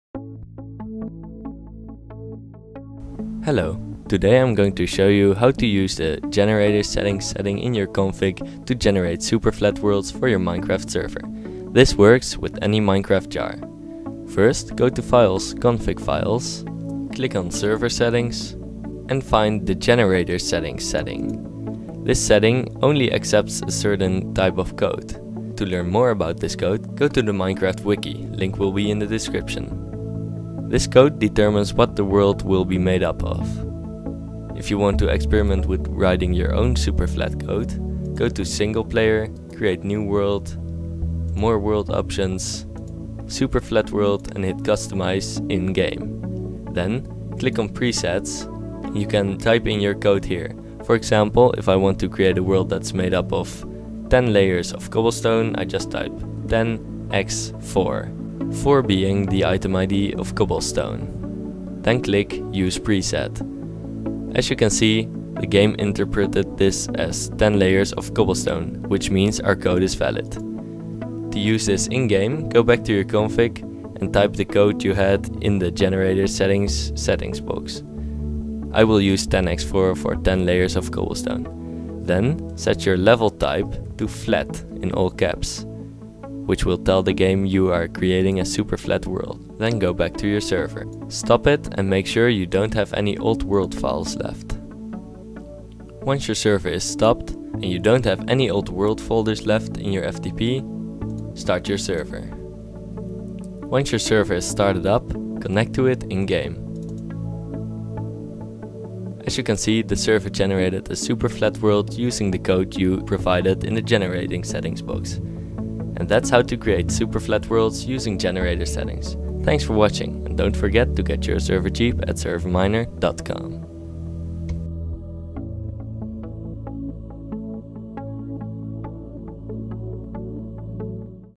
How to generate superflat worlds using the 'Generator-Settings' config setting
This tutorial will show you how to generate custom superflat worlds on your server using the 'generator-settings' setting in your server's config, and how to write the code that is needed to define these worlds.